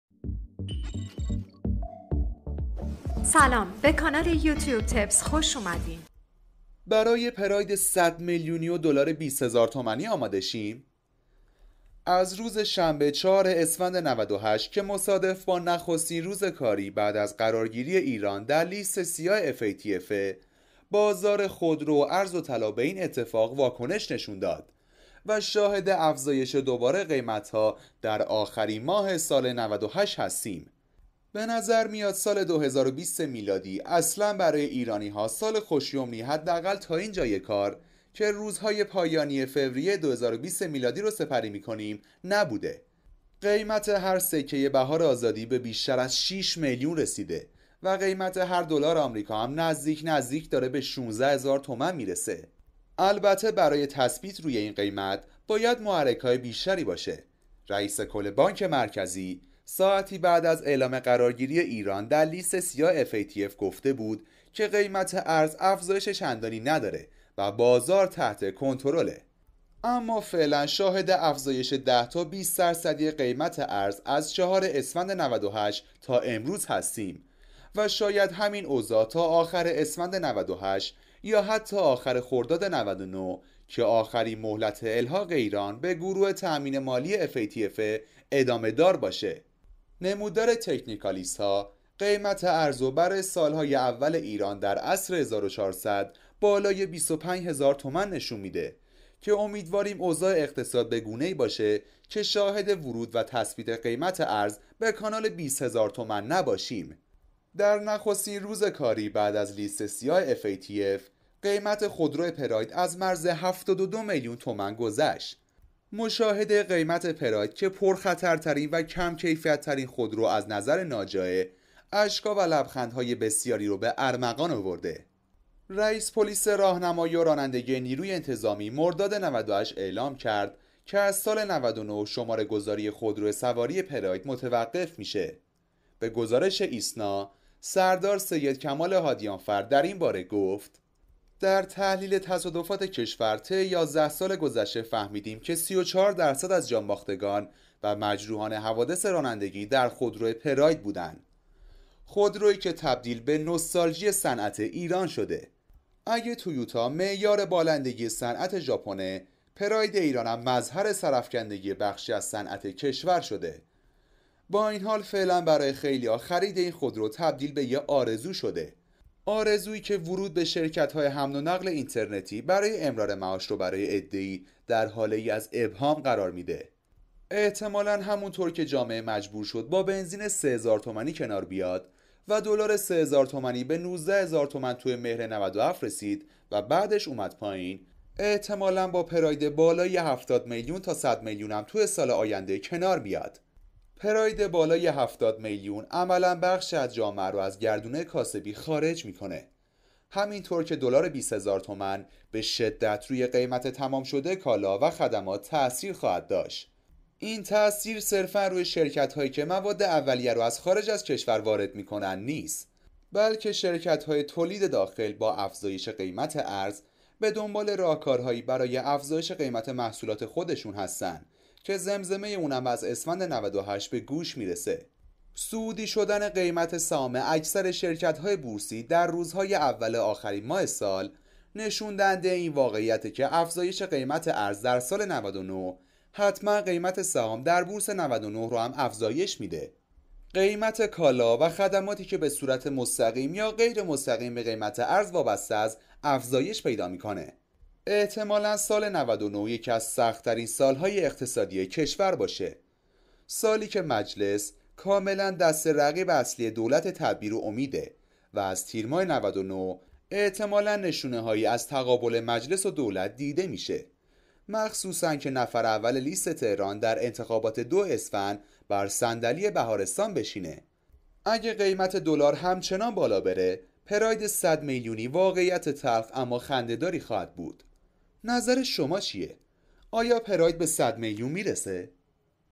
چرا باید برای پراید ۱۰۰ میلیونی و دلار ۲۰ هزار تومانی آماده شویم؟
برای پراید ۱۰۰ میلیونی و دلار ۲۰ هزار تومانی آماده شویم؟